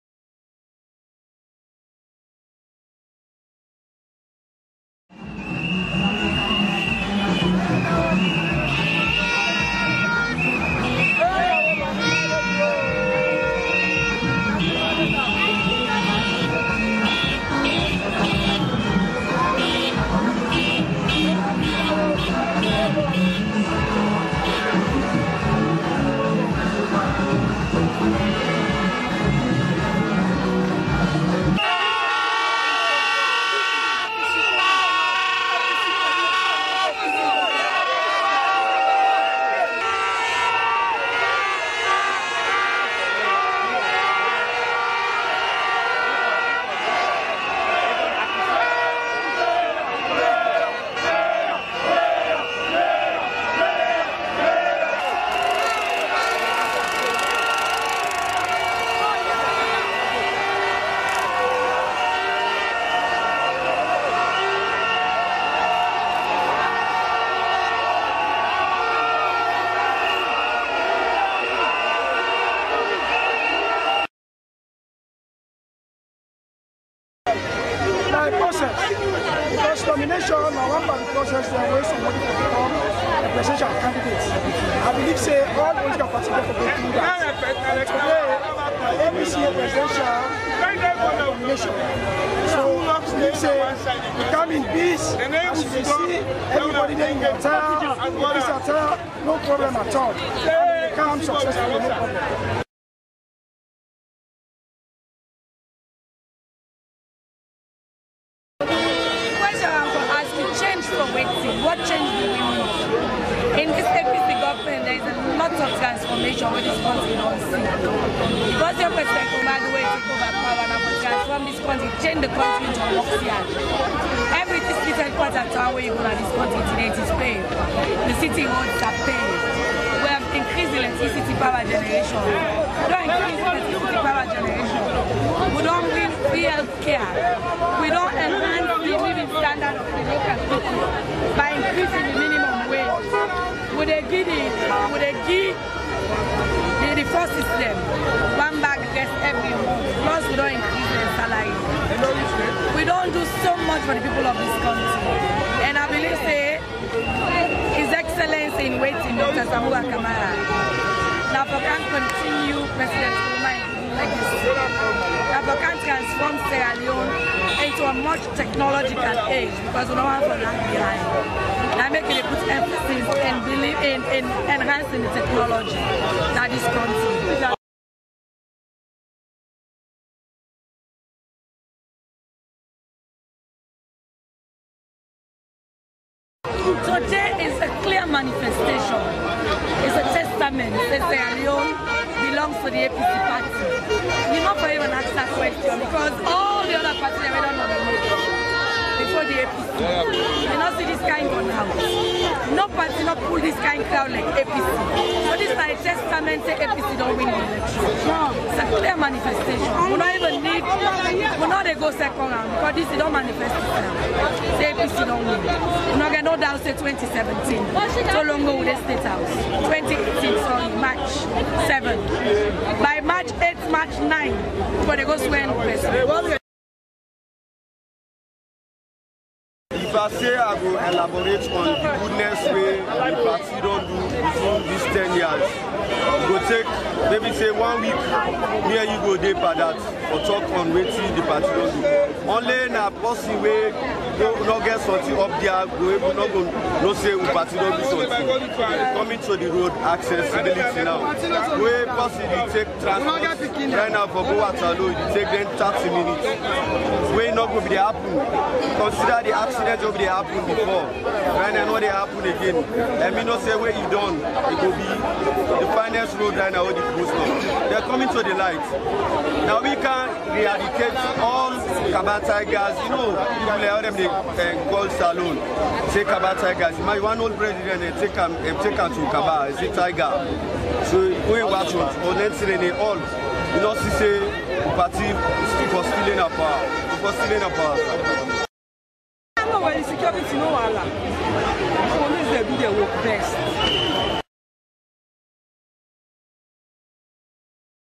All Peoples Congress NEC Presidential Nomination Day
On Friday 26 January 2018 the All Peoples Congress (APC) was at the National Electoral Commission Headquarters to do the official nomination of their Presidential Candidate Mr Samura Wilson Kamara and Vice Presidential Candidate Hon. Chernoh Ramadan Maju Bah (Chericoco)